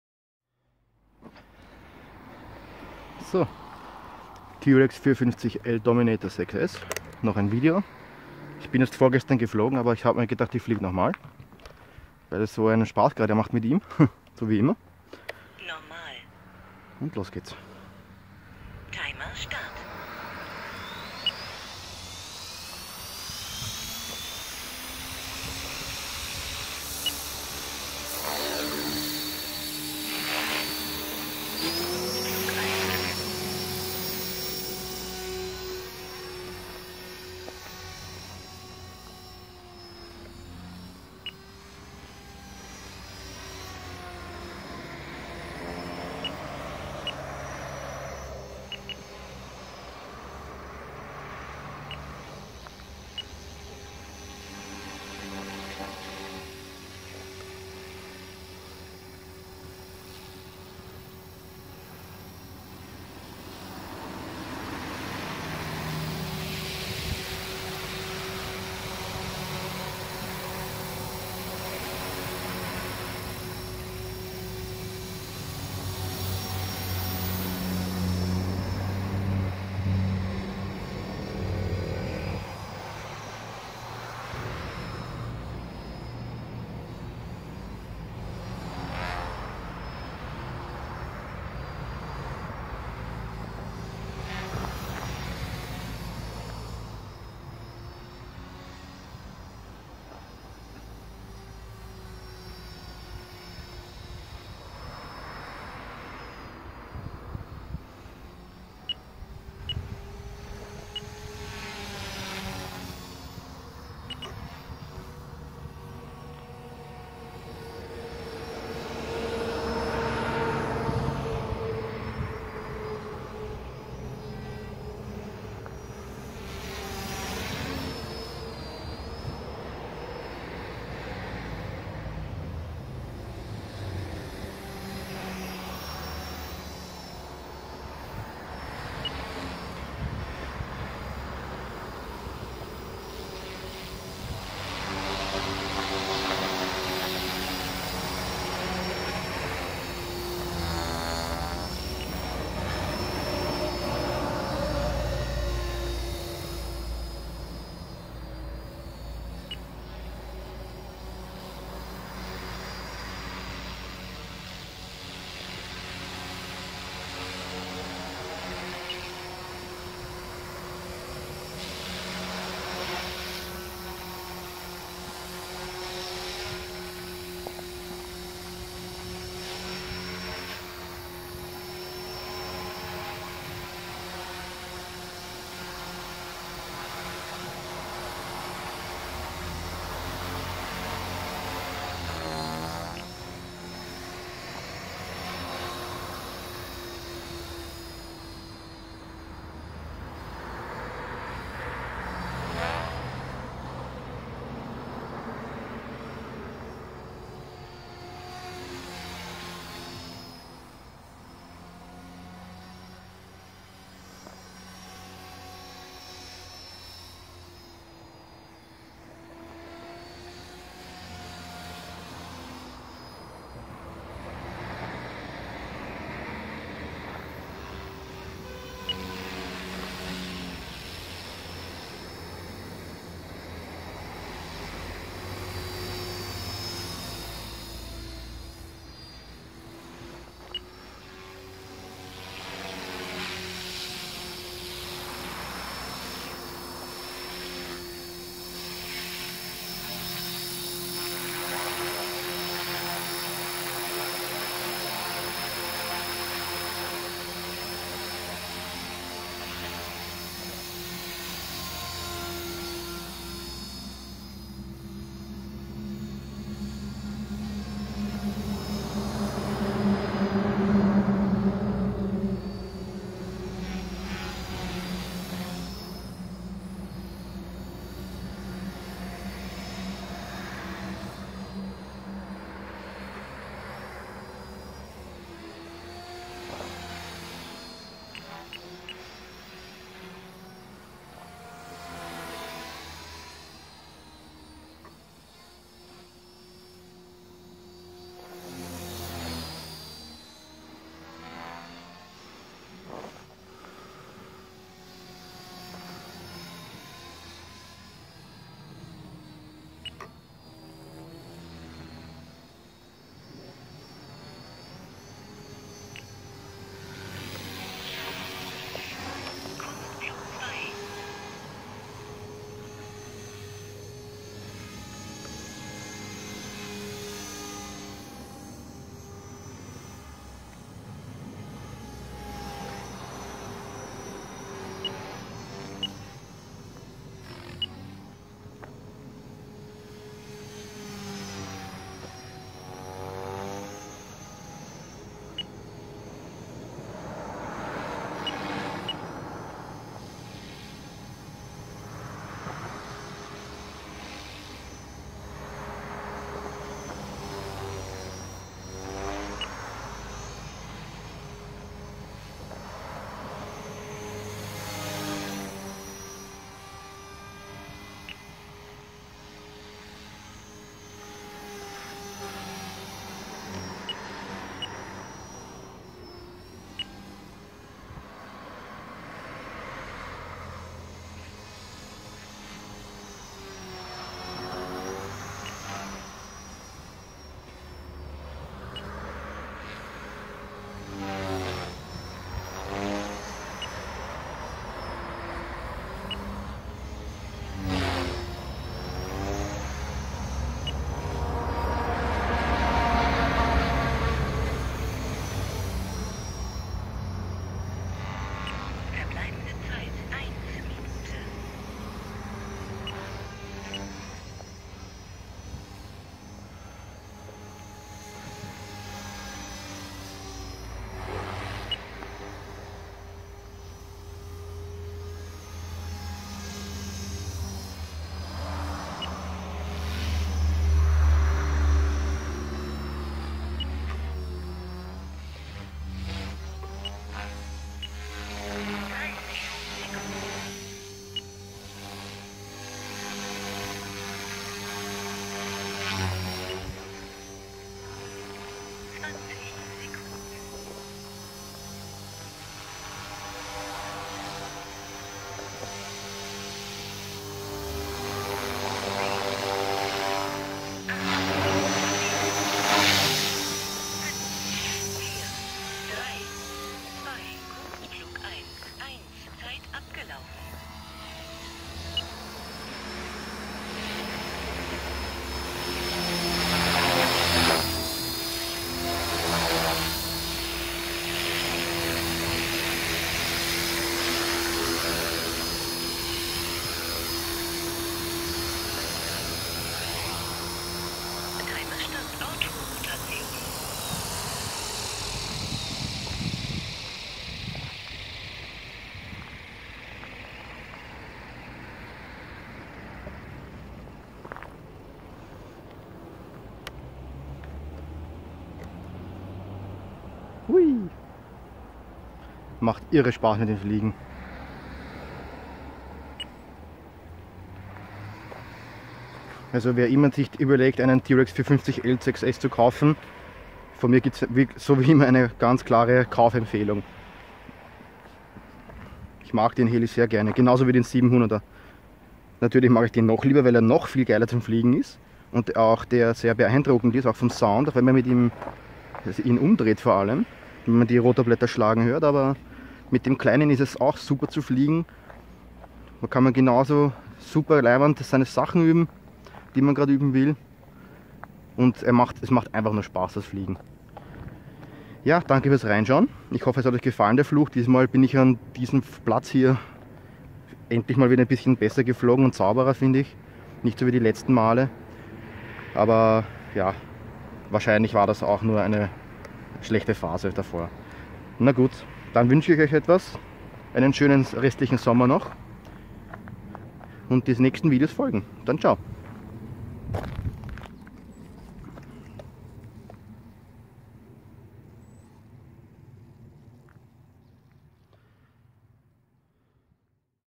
ALIGN T REX 450L Dominator 6S Flug 10
Hallo Helifreunde!

Hier wieder mal ein Video mit dem beliebten und echt geil fliegenden T-REX 450L Dom. 6S. Diesmal bin ich wieder bei dem kleineren Platz, wo ich mal wieder schönere Rundflüge in die Luft zeichne als die letzten paar Male.

Viel Spass beim Schauen, es folgt nachher auch wieder ein Video mit dem kleinen Angry Bird 180CFX ;-).